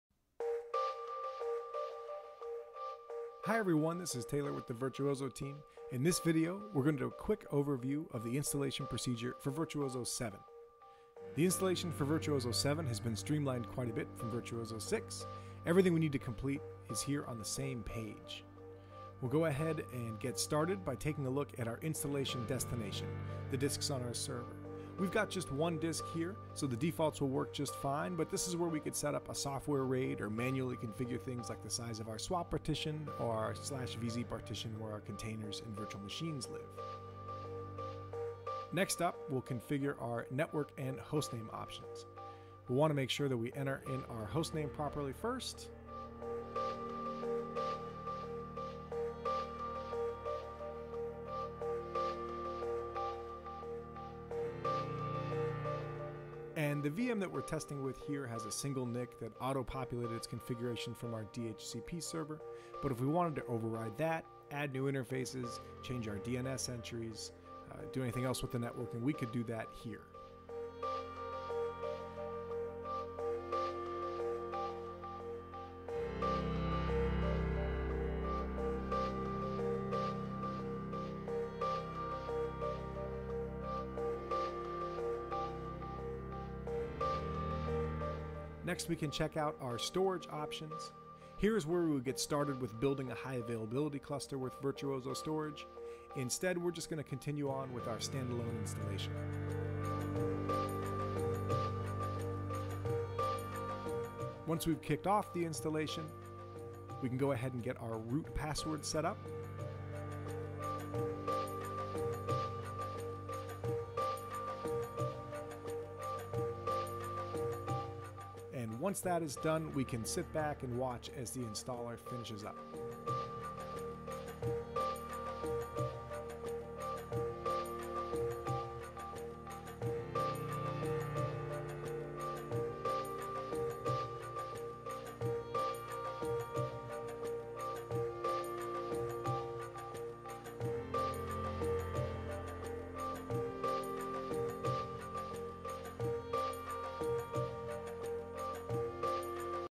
Virtuozzo 7 - Installation Guide
Get a quick overview of the installation procedure for Virtuozzo 7.